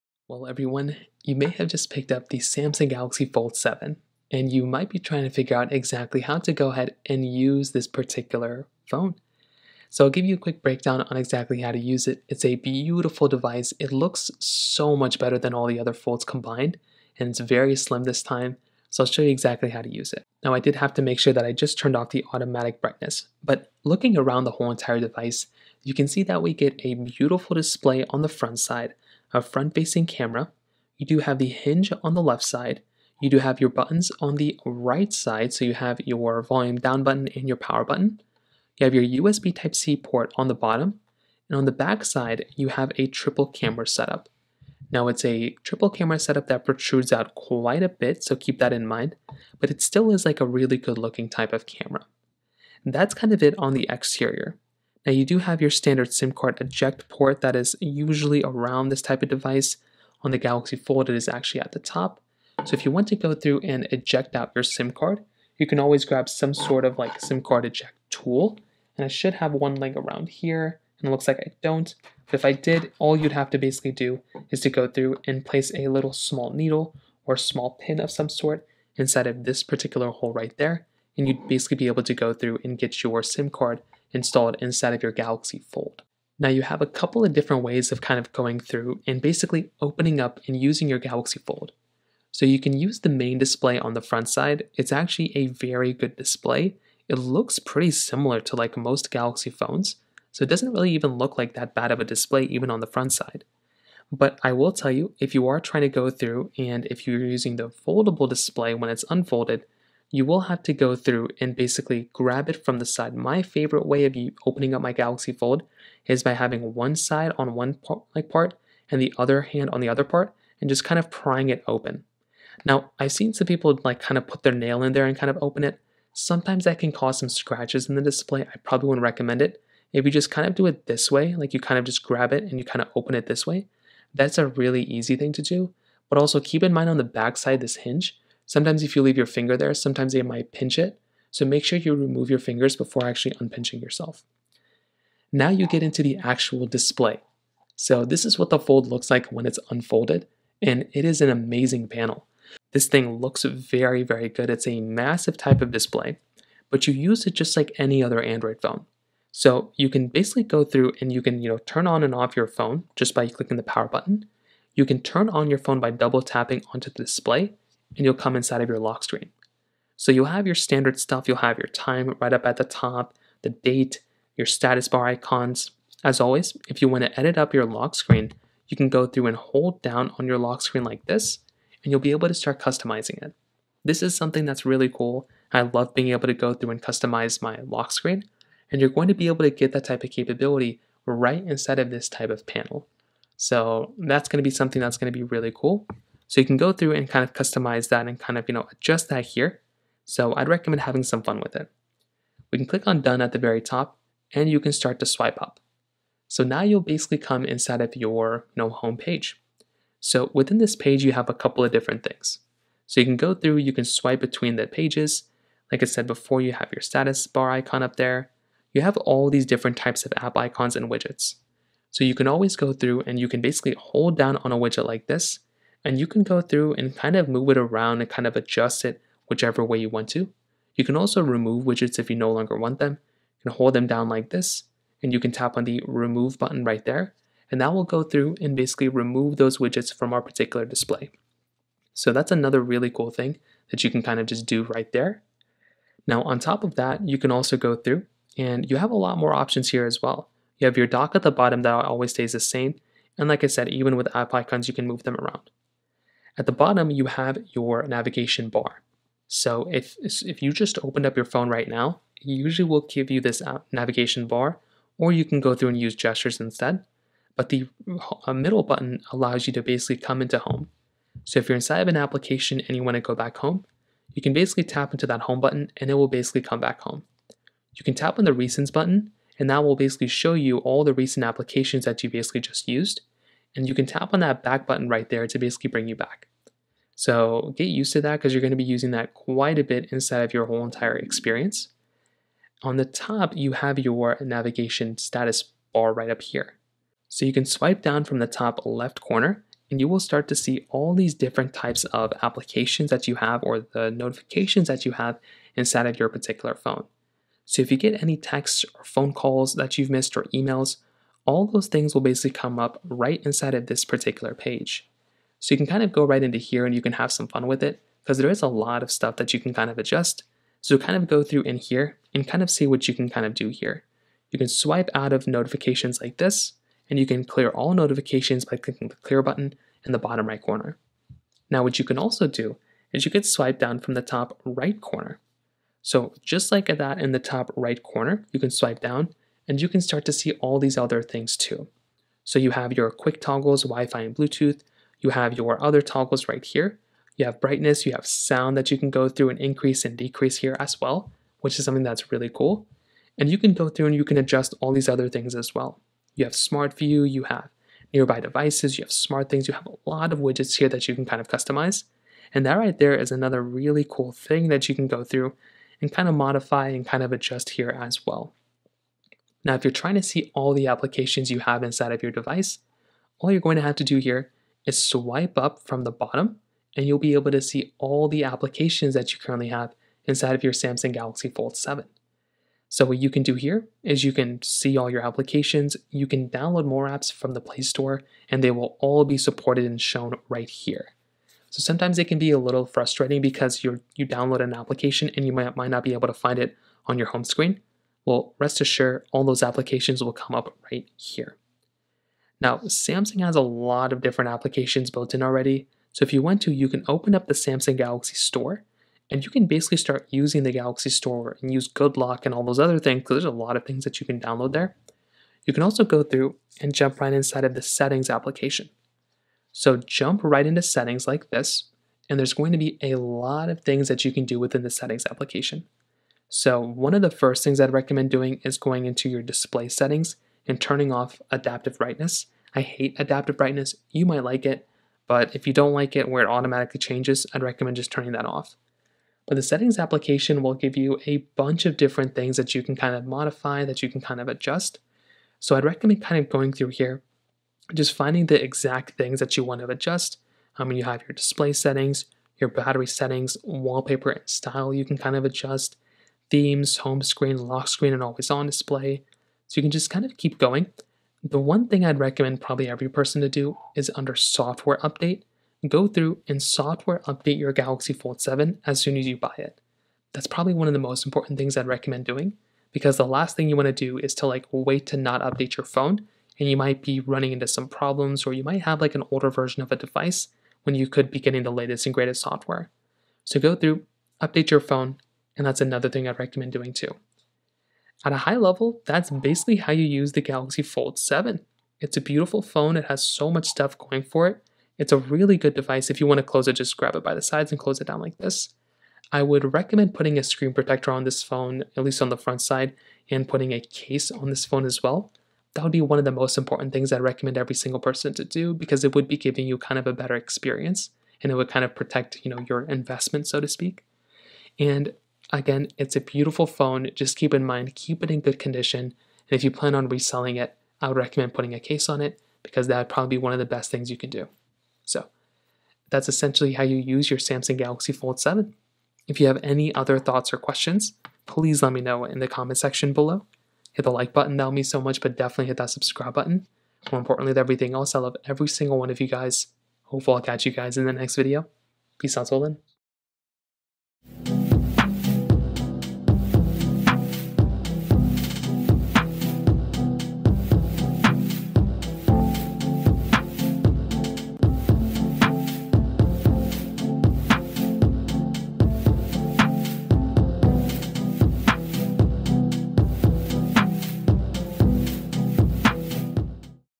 How To Use Samsung Galaxy Fold 7! (Complete Beginners Guide)
Let's take a look at the How To Use Samsung Galaxy Fold 7! (Complete Beginners Guide)